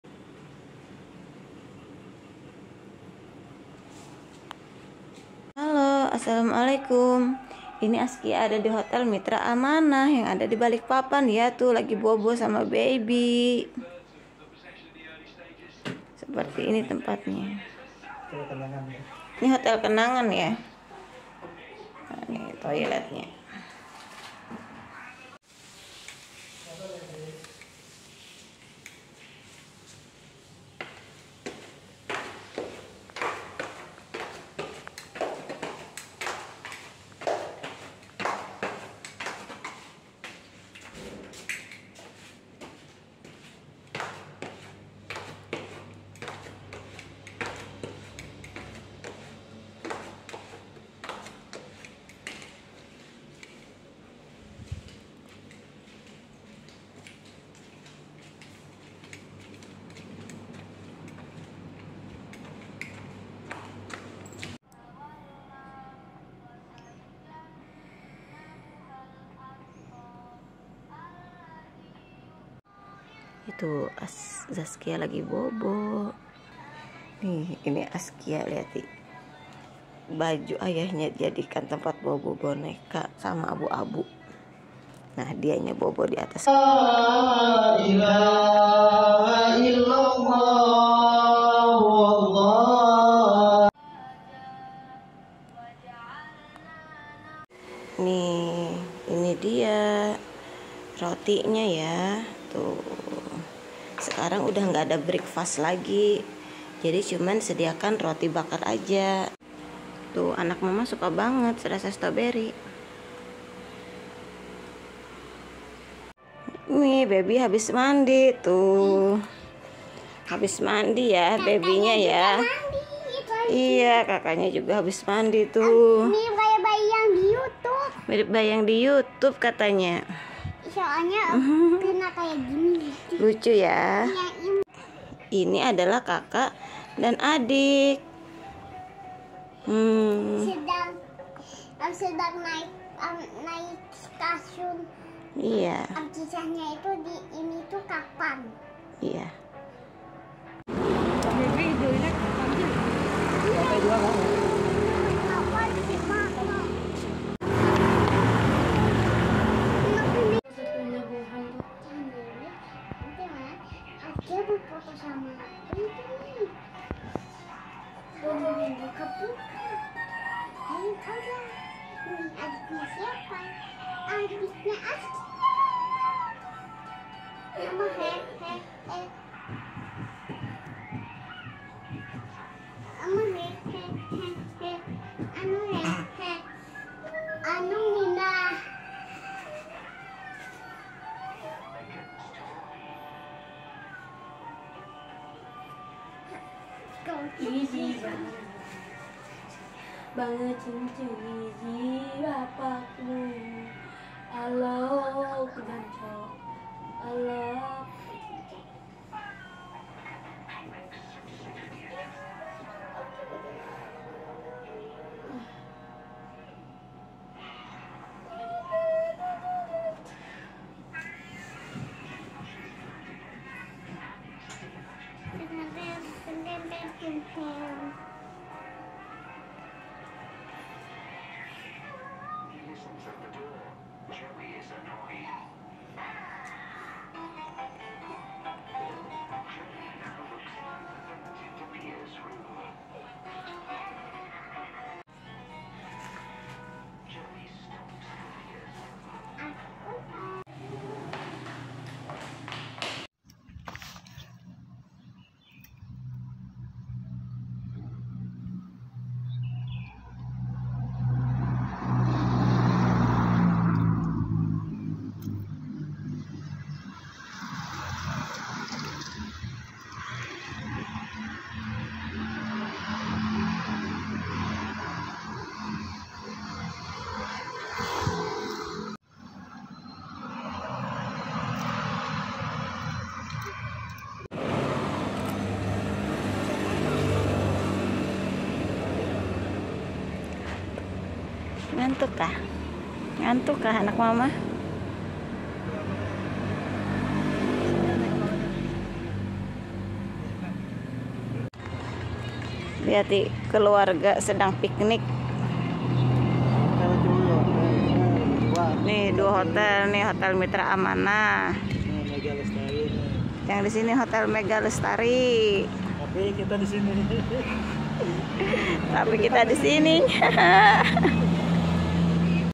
Hotel Mitra Amanah pertama kali baby zaskia ke sini
ini pertama kali Azkia dan baby zaskia ke sini
kalau Azka sudah dua kali
pertama tahun 2016 bersama alm mama mertua
harga hotel disini mulai 250 sd 350 rb